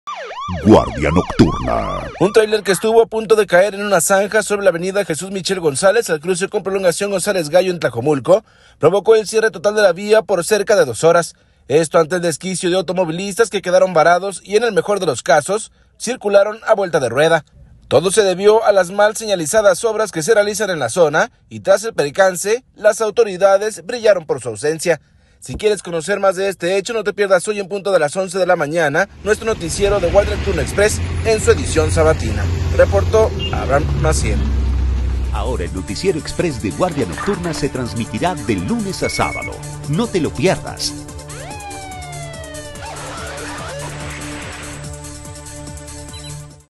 Un tráiler estuvo a punto de caer en una zanja sobre el cruce de Jesús Michel González y Prolonga
Un tráiler que estuvo a punto de caer en un impresionante socavón, terminó bloqueando los carriles centrales de la avenida Jesús Michel González; antes de Periférico Sur